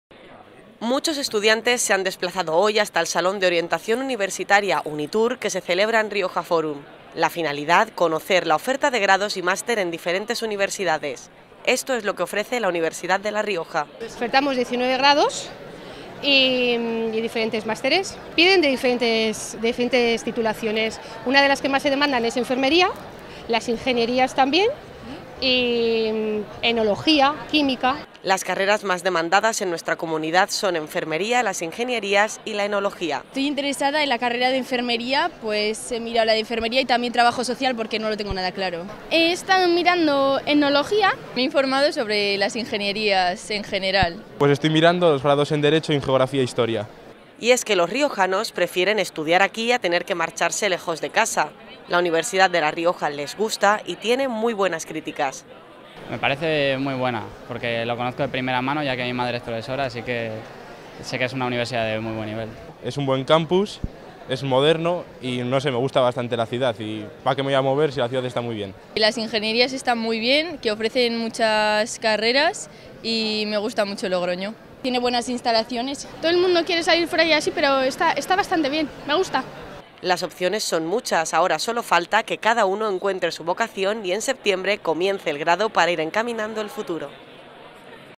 La Universidad de La Rioja en la Feria Unitour 2017
La Universidad de La Rioja presenta su oferta académica para el curso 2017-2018 en el Salón de Orientación Universtaria 'Unitour 2017'.